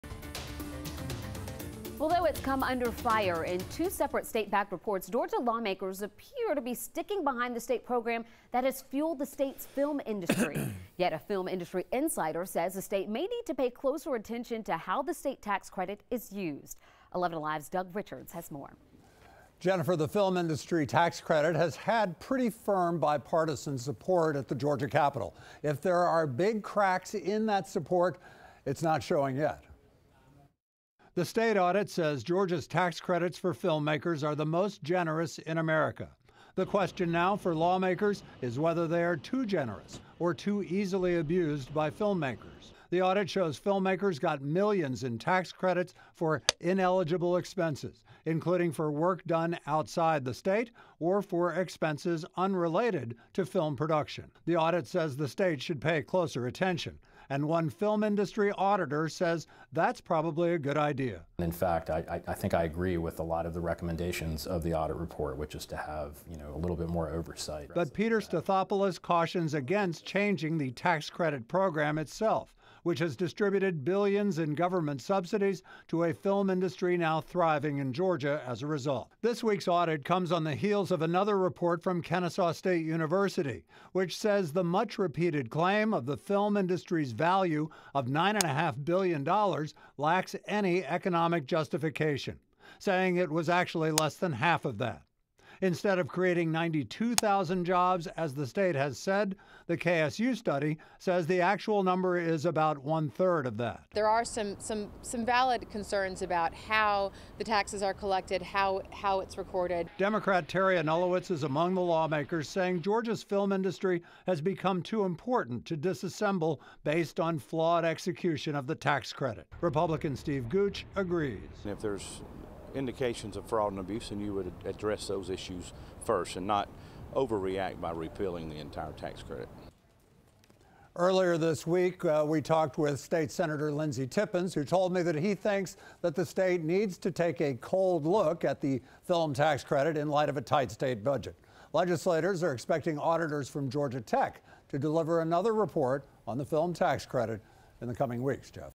New audit: Too much tax money paid to GA film industry
The state audit says Georgia grants more tax credits than any other state in America